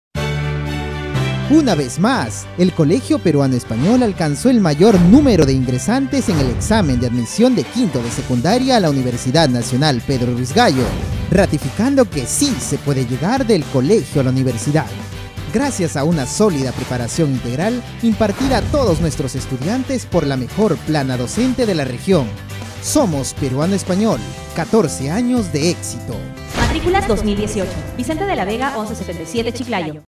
SPOT PERUANO ESPAÑOL INGRESANTES 2017
Presentamos nuestros ingresantes a la UNPRG en el examen de 5to de secundaria 2018 - I